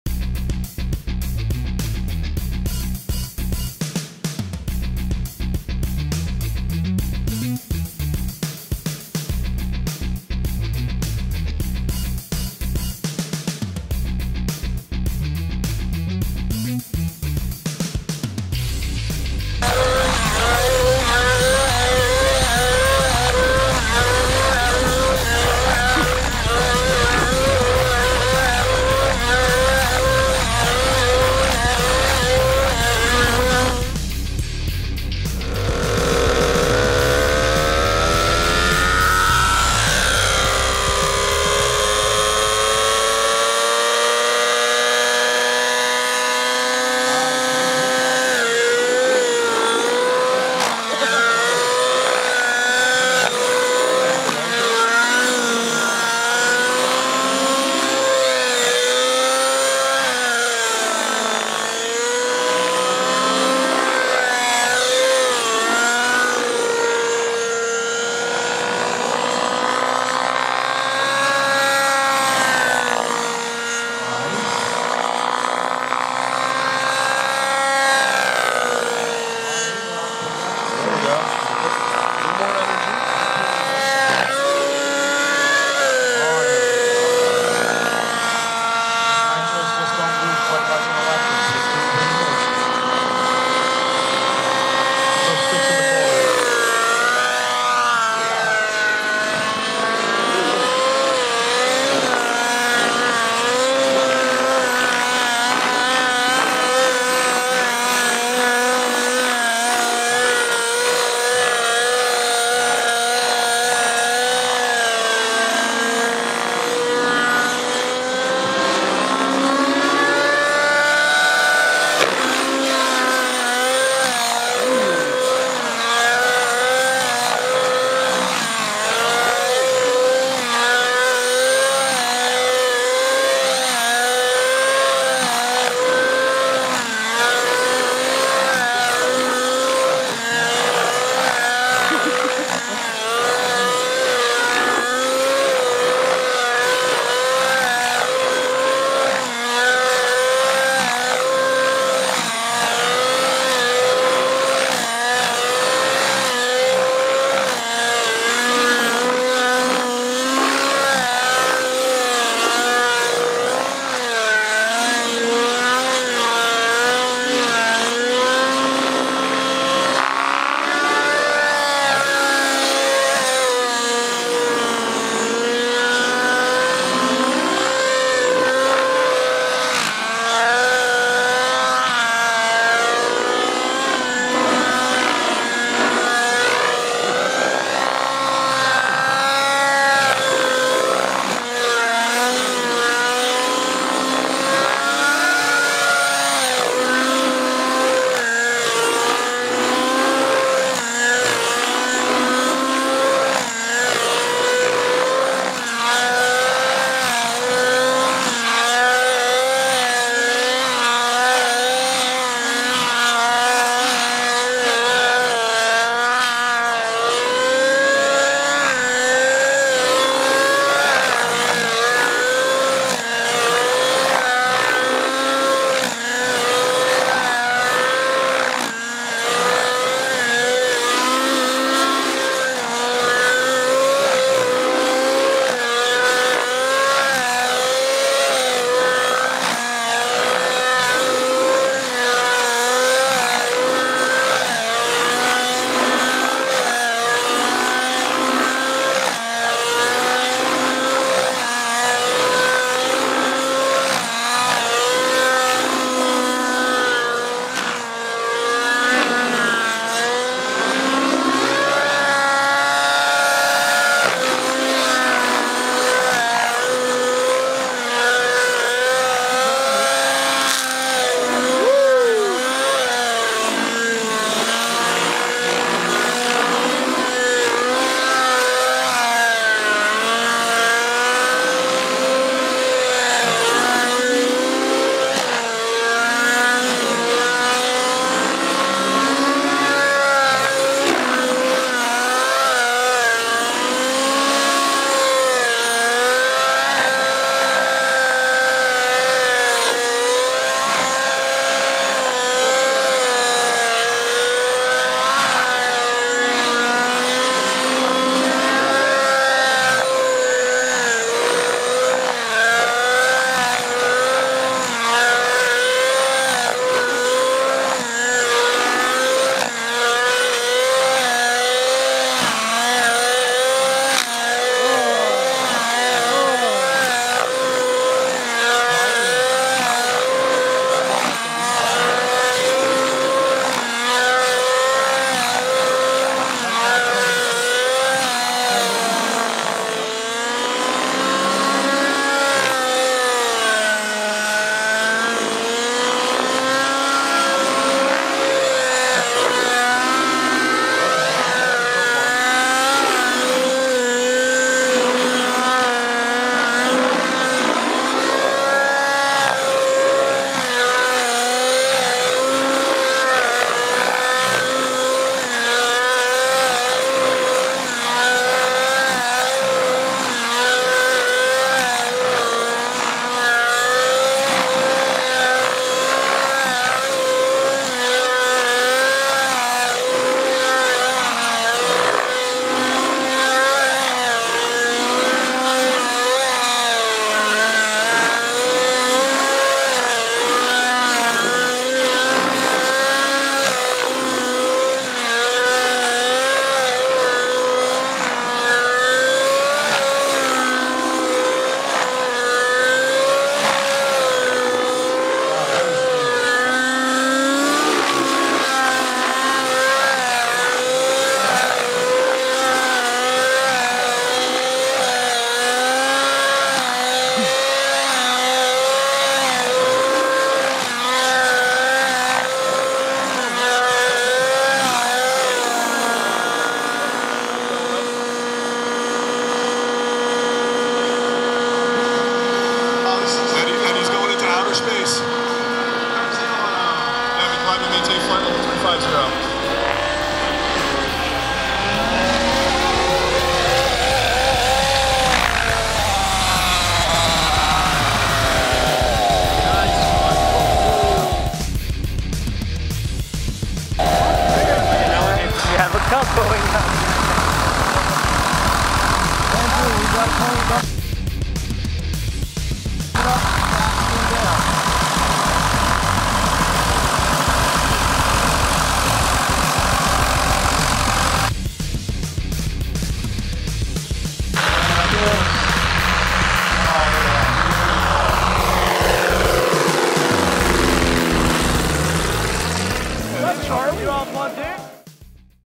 Devin McClellan flying MA Whiplash Nitro
Watch Devin McClellan's incredible flight over the beans of South Jersey!    Down close, during the golden hour, Devin pushes the limits.    EPIC flight with the Miniature Aircraft  Whiplash Nitro!